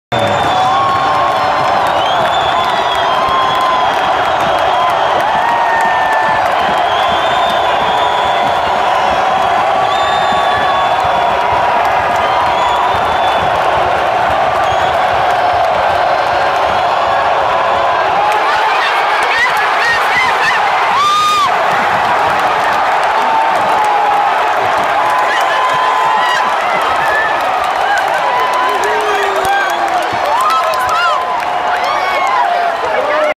Century Link Field
Seattle Seahawks home game against the Dallas Cowboys, October 12, 2014